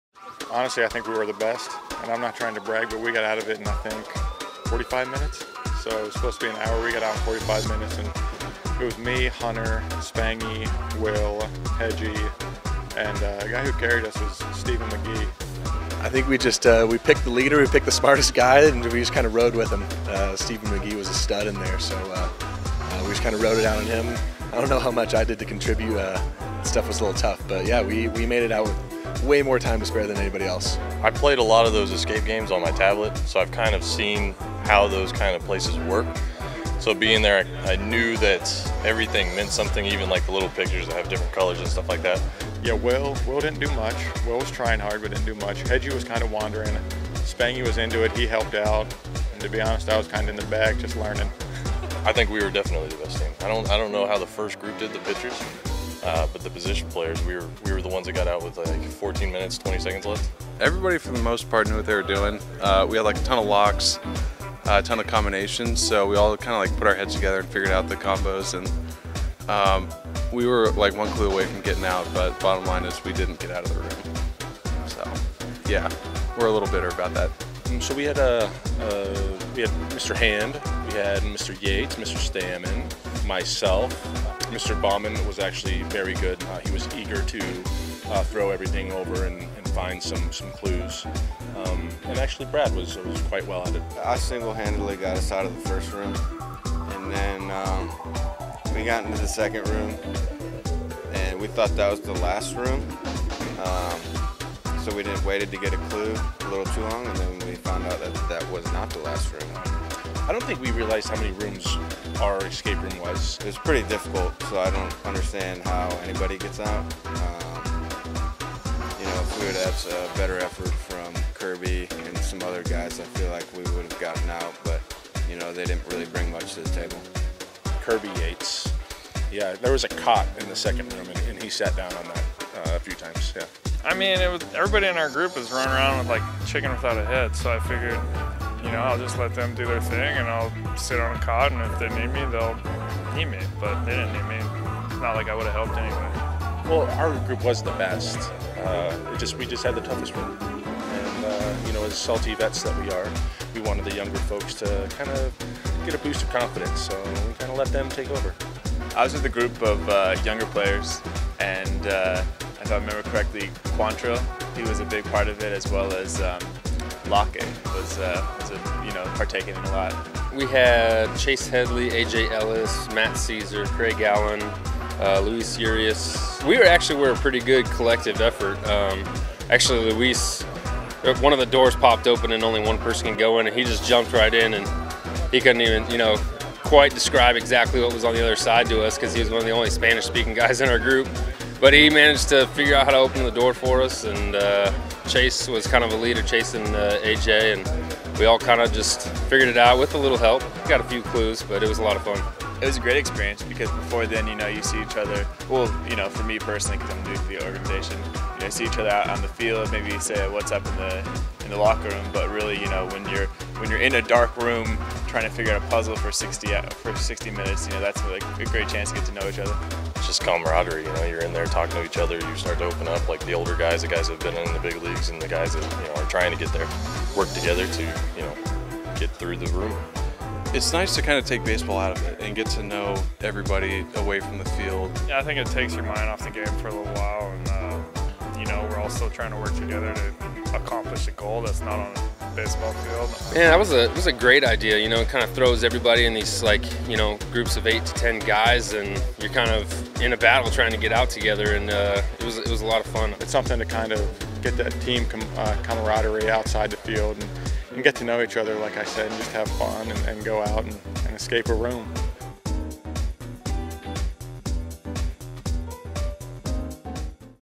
Padres Try An Escape Room | San Diego Union-Tribune
As part of a team bonding exercise, Padres manager Andy Green took his team to the Escape Room in Arizona. Players were split up into groups of 8-10 and tasked with getting out. Hear from the players who made it ... and who didn't.